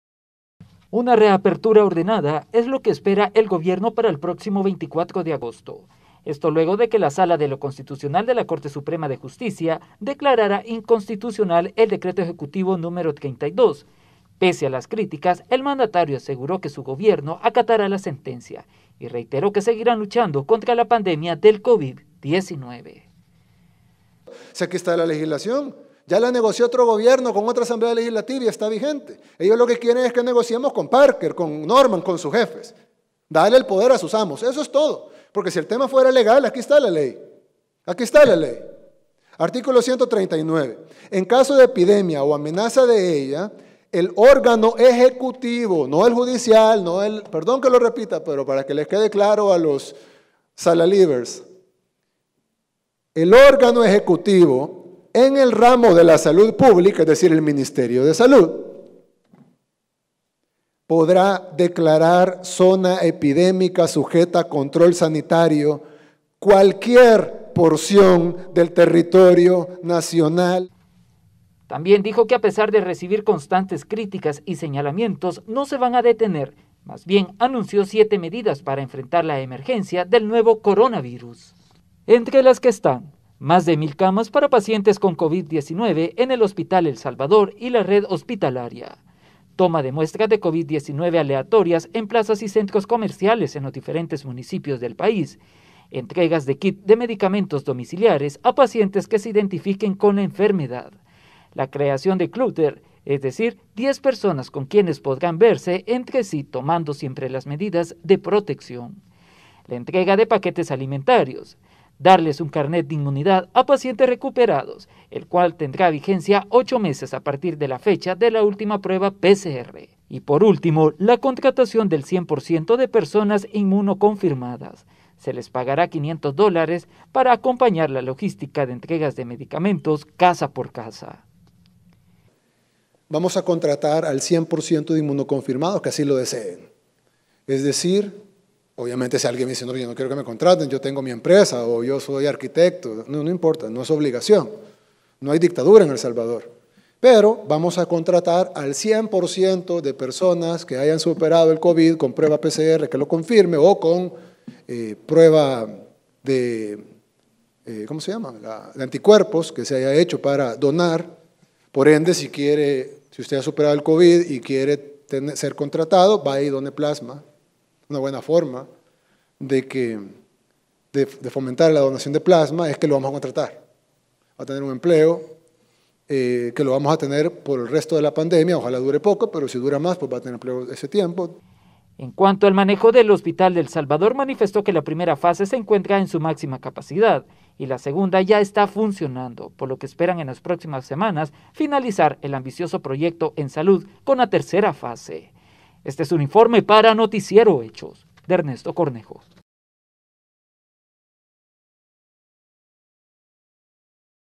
Gobierno dice que acatará resolución de la Sala de lo constitucional
El presidente Bukele anunció que las personas que posean este documento serán contratadas para realizar labores de logística para el Gobierno.

¡Visítenos!